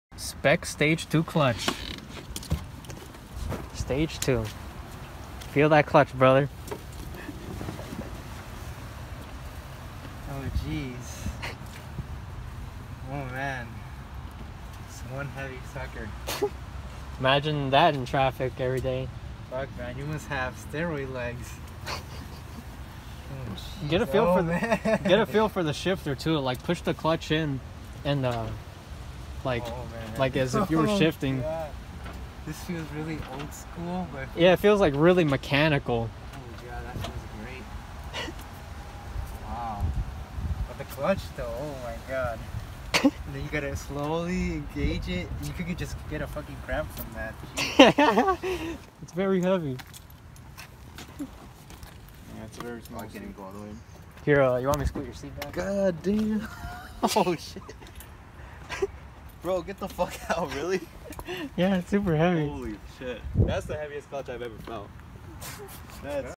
Spec Stage 2 Heavy Clutch Pedal Reaction 2001 Mustang GT
Just some funny reactions to my heavy Spec Stage 2 Clutch pedal that I use daily on my 2001 Mustang GT.  It’s not too bad honestly, it’s a nice feel if you’re into the heavy clutch pedal. Still can bang gears quickly. Parking can suck sometines haha. Stock steel flywheel. 2v mustang gt stiff heavy racing clutch spec.